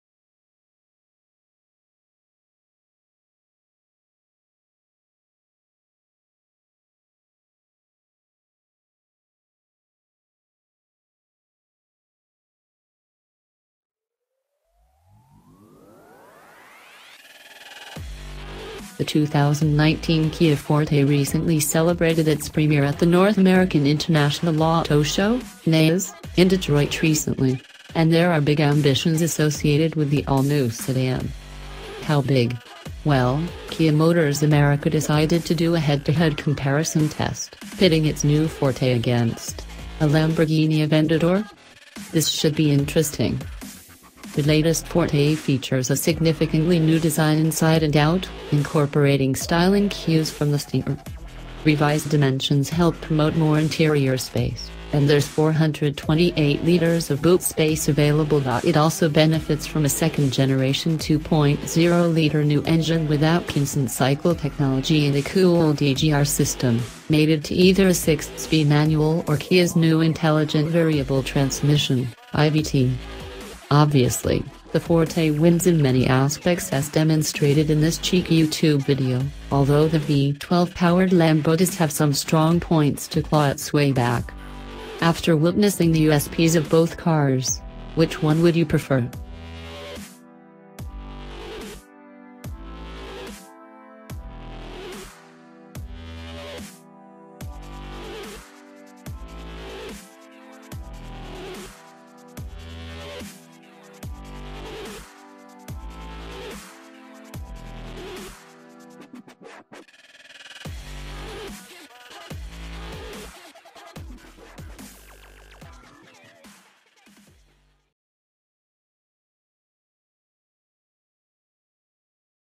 Hot News !!! Lamborghini Aventador vs 2019 Kia Forte
Hot News  !!! Lamborghini Aventador vs  2019 Kia Forte - The 2019 Kia Forte recently celebrated its premiere at the North American International Auto Show (NAIAS) in Detroit recently, and there are big ambitions associated with the all-new sedan.

How big? Well, Kia Motors America decided to do a head-to-head comparison test, pitting its new Forte against, a Lamborghini Aventador? This should be interesting.

The latest Forte features a significantly new design inside and out, incorporating styling cues from the Stinger. Revised dimensions help promote more interior space, and there’s 428 litres of boot space available.It also benefits from a second-generation 2.0 litre Nu engine with Atkinson-cycle technology and a cooled EGR system, mated to either a six-speed manual or Kia’s new Intelligent Variable Transmission (IVT).

Obviously, the Forte wins in many aspects as demonstrated in this cheeky YouTube video, although the V12-powered Lambo does have some strong points to claw its way back. After witnessing the USPs of both cars, which one would you prefer?

██████ GET DAILY UPDATES! NOW! ██████